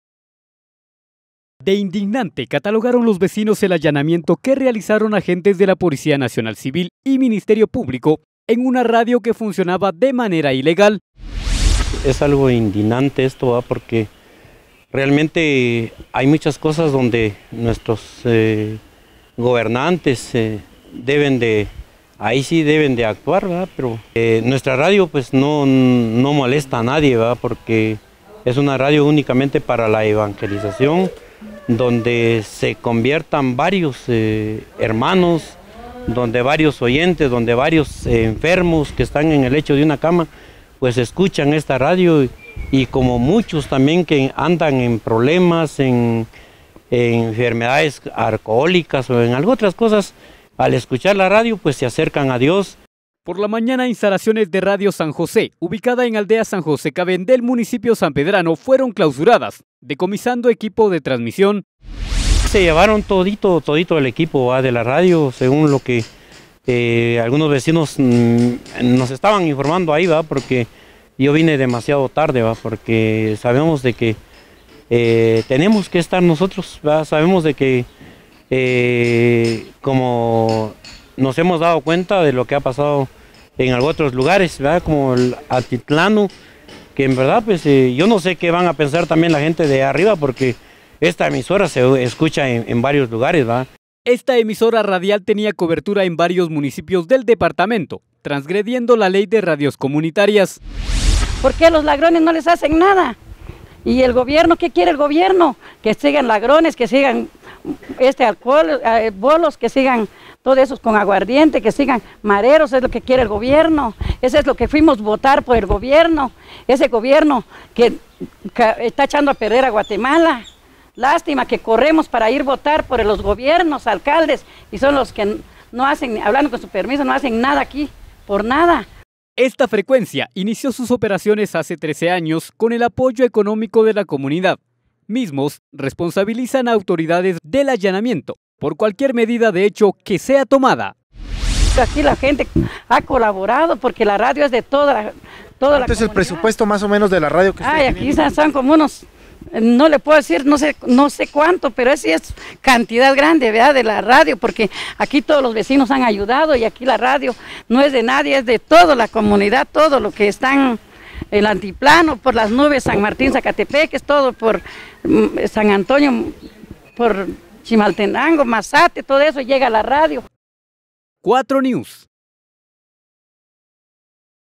Vecinos inconformes por allanamientos en San José Caben.
De Indignante catalogaron los vecinos el allanamiento  que realizaron agentes de la Policía Nacional Civil y  Ministerio Publico en una radio que funcionaba de manera ilegal.